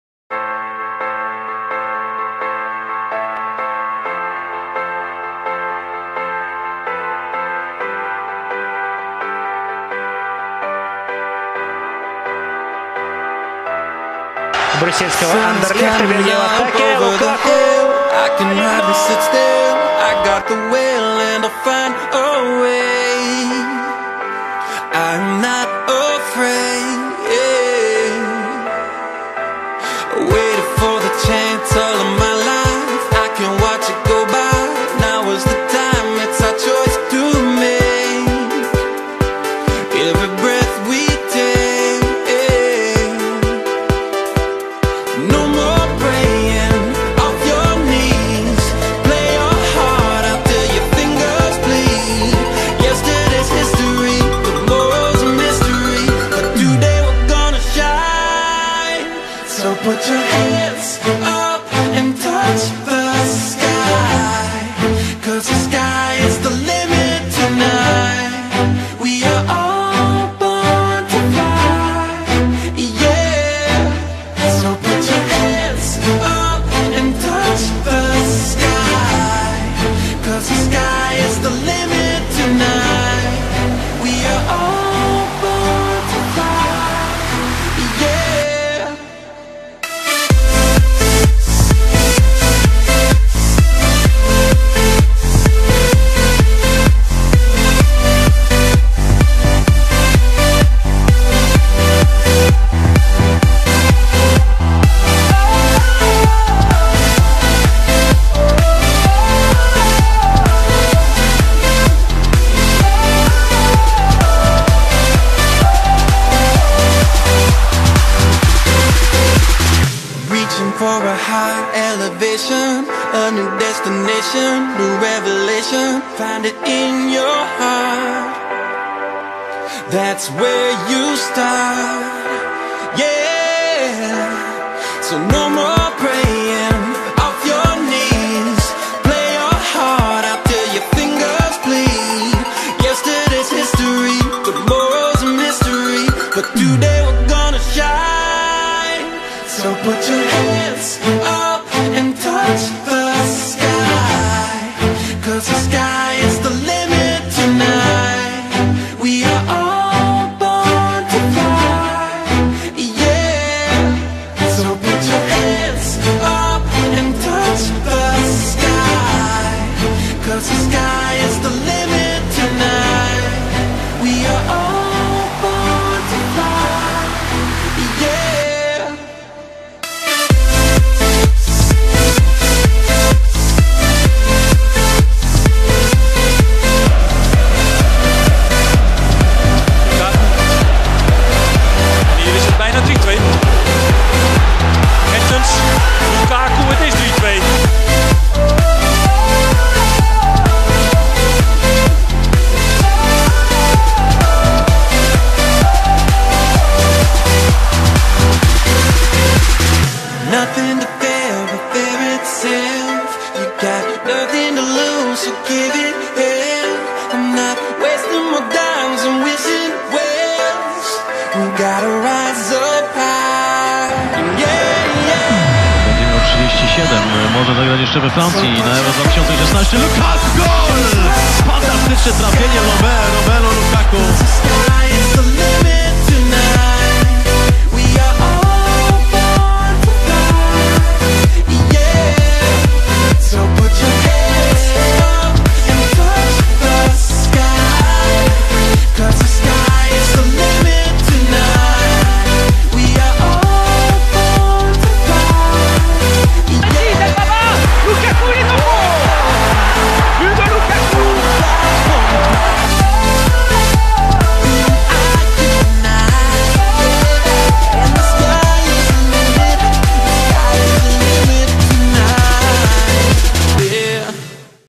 Romelu Lukaku ►New Drogba | 2014
Romelu Menama Lukaku (born 13 May 1993) is a Belgian footballer who plays as a striker for Premier League club Chelsea and the Belgium national team. He started his career with local side Rupel Boom, before being scouted and joining Lierse. He played well and attracted many clubs to his talents. He alerted Anderlecht and signed for the Belgian Pro League side in 2006.

Lukaku continued his development with Anderlecht and made his professional debut while still at school at the age of 16. Whilst playing for Anderlecht, he became the 2009--10 top scorer in Belgium as Anderlecht won the Belgian championship, and won the Belgian Ebony Shoe in 2011. In the 2011 summer transfer window, Lukaku joined English Premier League club Chelsea for an undisclosed fee, signing a five-year contract with the London club.

In January 2014, he was named by The Guardian as one of the ten most promising young players in Europe.